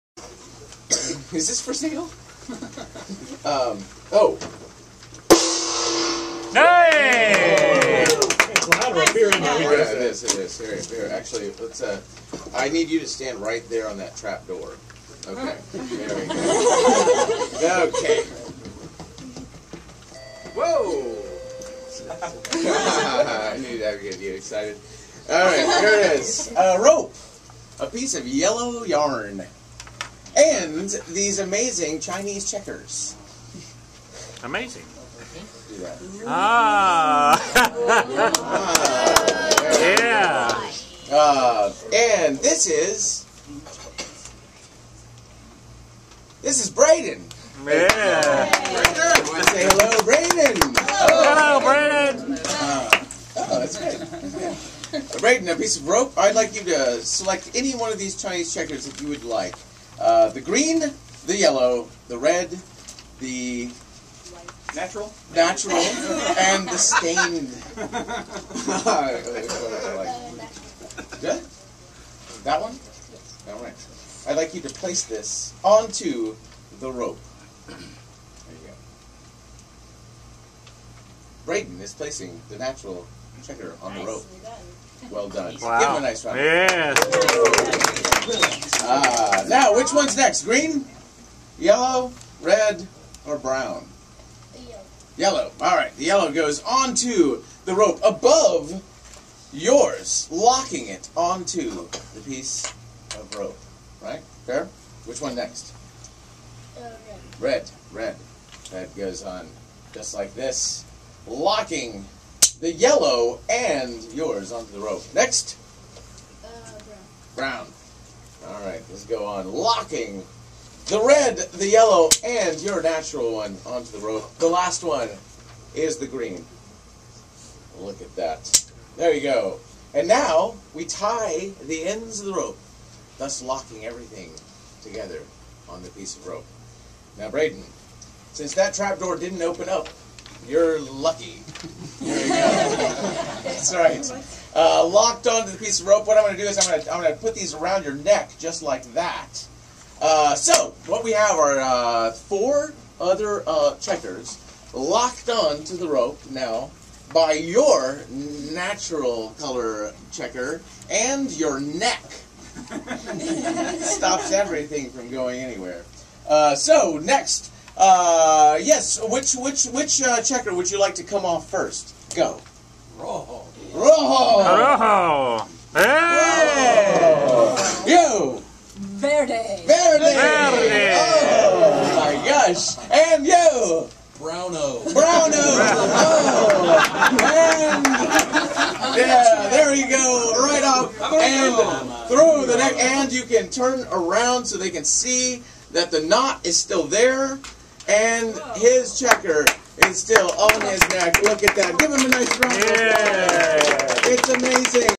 Checker Necklace - Louie Gaynor
Special thanks for Magic ETC., Derrek Kennedy - Fort Worth, Texas, for allowing us to feature this great demo shot at Magic ETC.

This unit hand-crafted by Louie Gaynor, made with larger sized checkers for greater visability and a sturdy stand for security during transport.